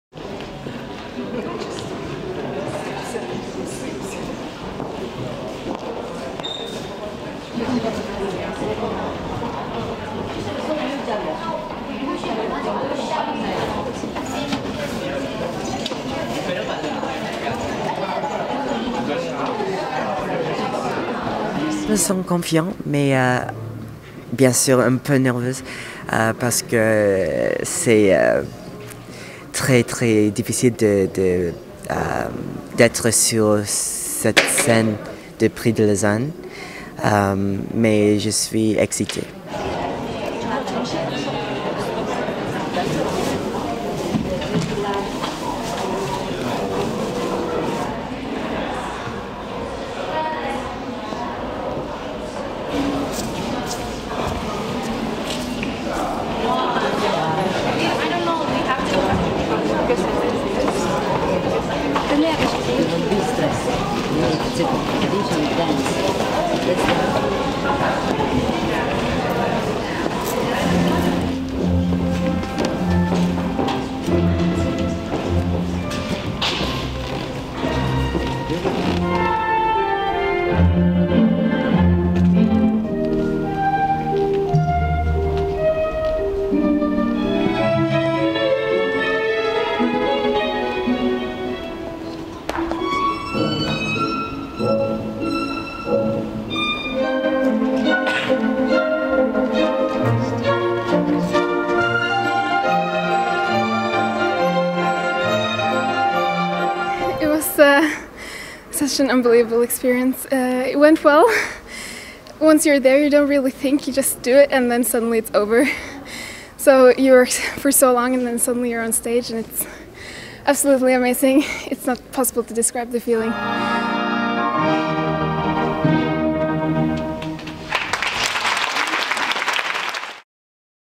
Dans les coulisses du Prix de Lausanne
Une septentaine de jeunes danseurs venus du monde entier et représentant une quinzaine de pays participent à la 46e édition du Prix de Lausanne, qui a débuté lundi. Samedi se tiendra la finale. Quelque vingt jeunes privilégiés pourront y prendre part. Petit tour des coulisses lors des sélections du groupe 14-17 ans, vendredi après-midi au Théâtre de Beaulieu.